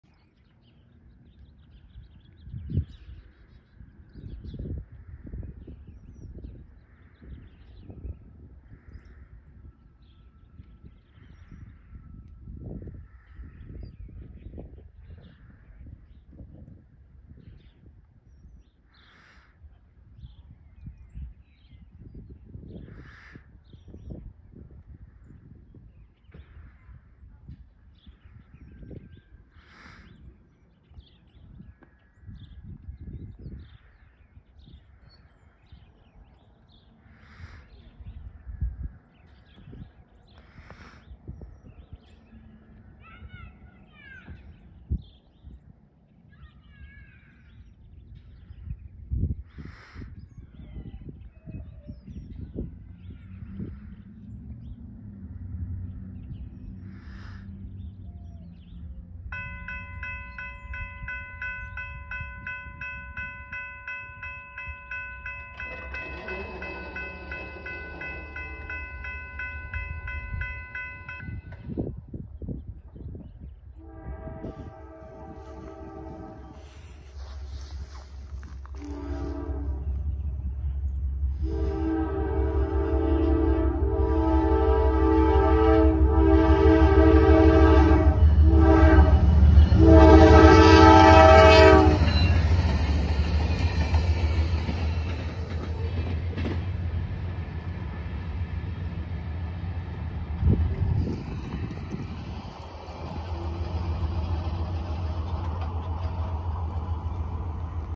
Norfolk Southern 904 in Goldsboro - 2/28/22
Norfolk Southern geometry train 904 is seen crossing Broadway Street on the Port Road Branch with a Nathan K5LA horn in Goldsboro, Pennsylvania at 3:55 PM on Monday, February 28, 2022. The engines in this video are: NS 9647 (D9-44CW) and NS 34 (Geometry Car). Enjoy!